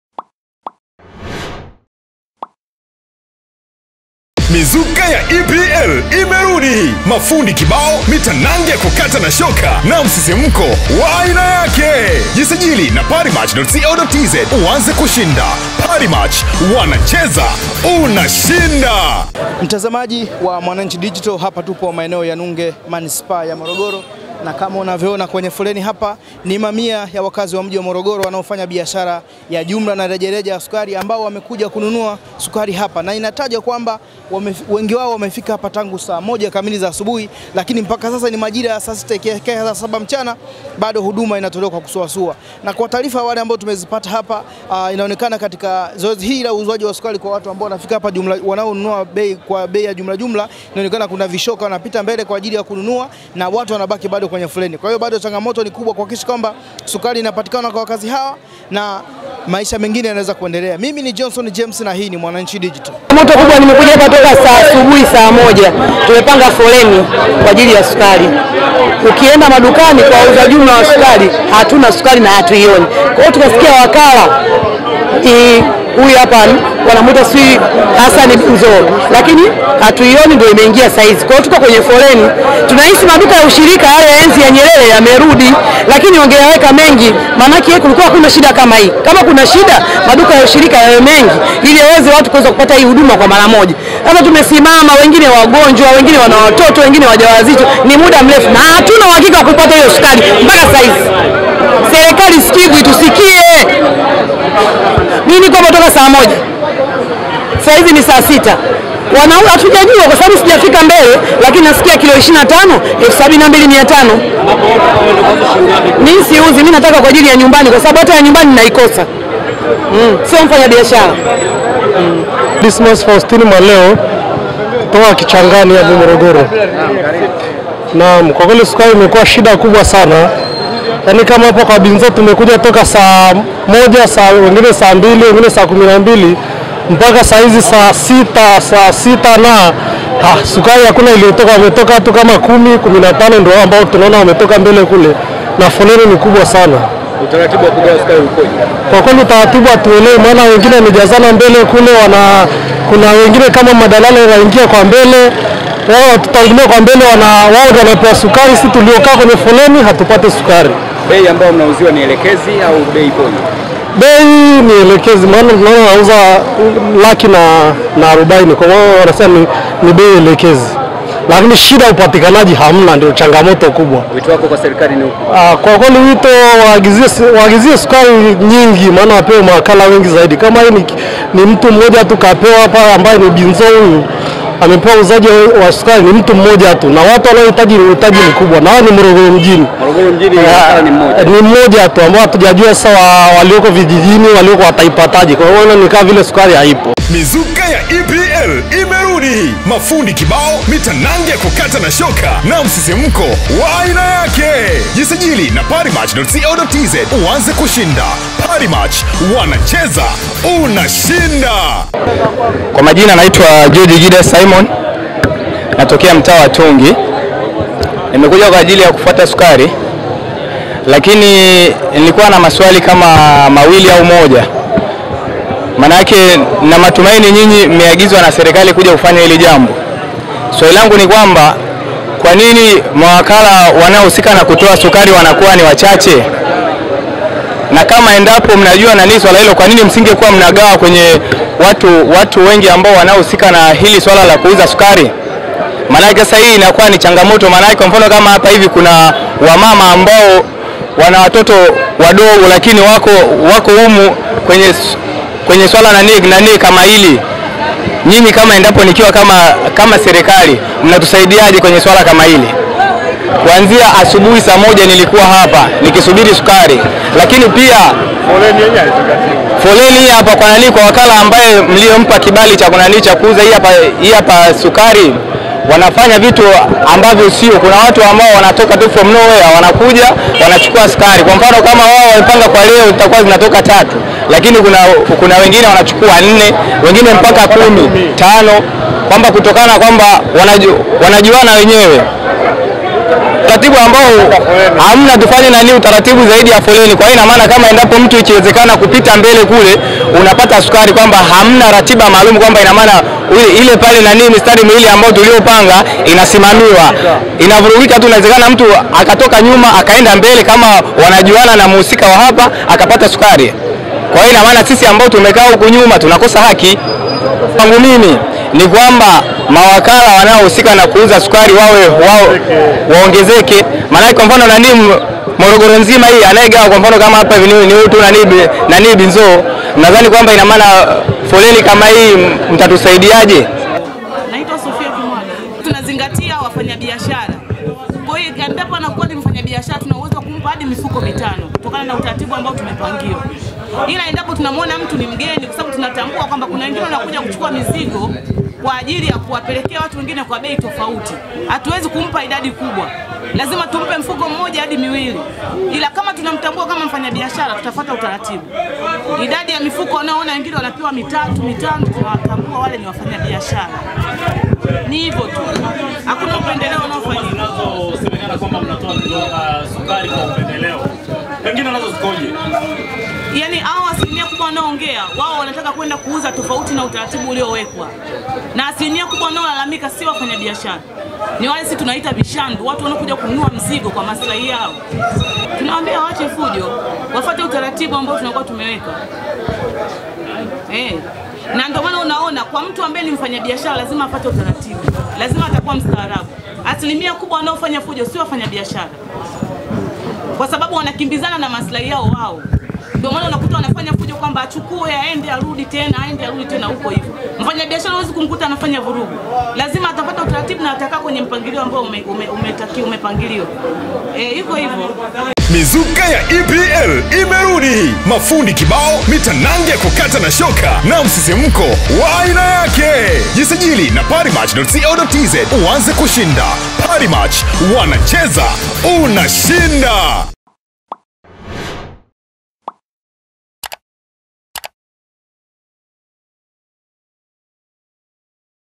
Wafanyabiashara Morogoro wanunua sukari kwa foleni
Cheza sasa kupitia na ushinde

Wafanyabiashara wa sukari Manispaa ya Morogoro wamelazimika kupata sukari kwa kupanga foleni leo Februari 26, 2024 baada ya uuzwaji kuanza kusuasua.

Mkoa wa Morogoro ambao huchangia upatikanaji wa sukari nchini, wakazi wake wameendelea kutabika kupata bidhaa hiyo.

Wakizungumza na Mwananchi Digital, wafanyabiashara wa sukari wameeleza namna wanavyopata changamoto kununua bidhaa hiyo muhimu.